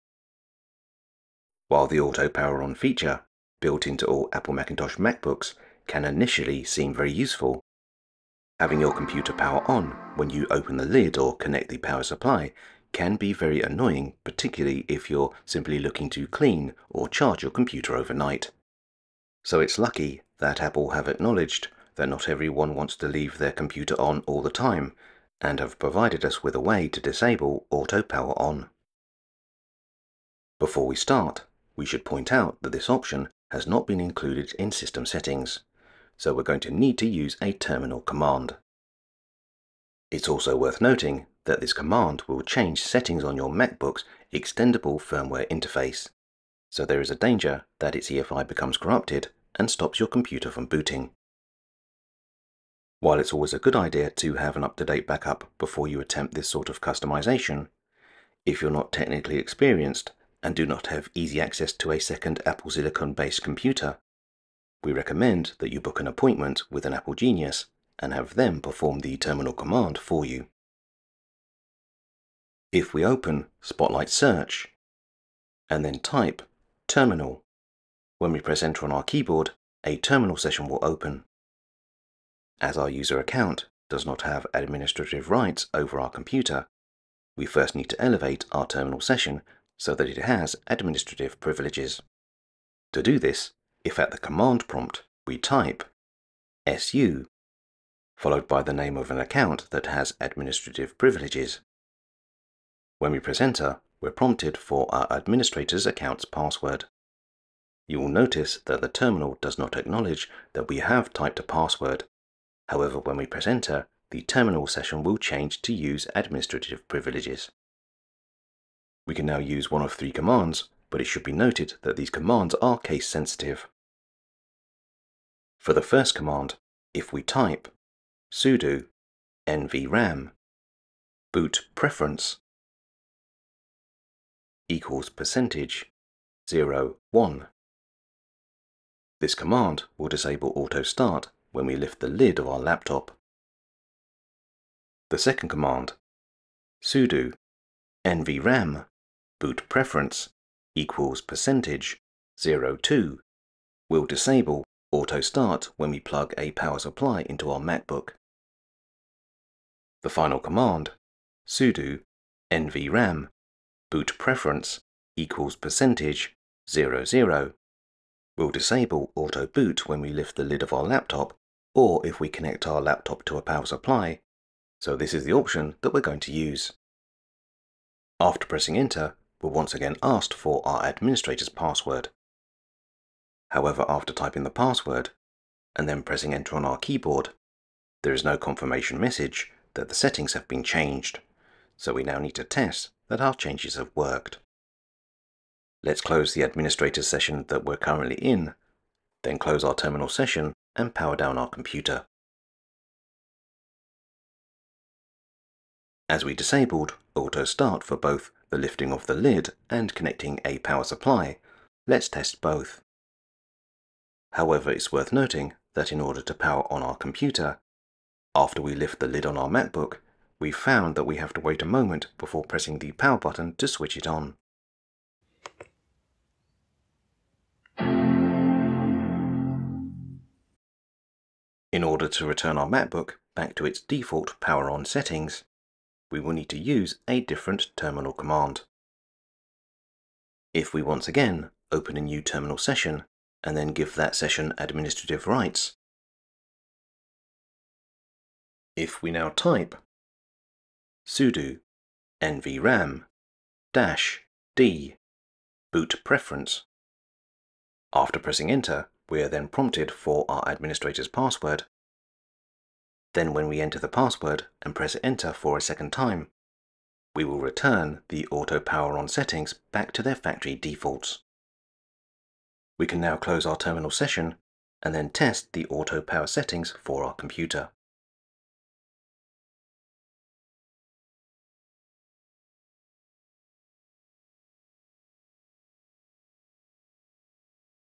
How to disable auto power on an Apple MacBook
While the auto power on feature built into all Apple Silicon MacBooks can initially seem very useful. Having your computer power on when you open the lid or connect a power supply, can be very annoying. Particularly if you are simply looking to clean or charge your computer over night.

So it’s lucky that Apple have acknowledged that not everyone what’s to leave their computer on all the time and have provided us with a way to disable auto power on. However, as we will have to type a command into the terminal, this video demonstrates what you need to do.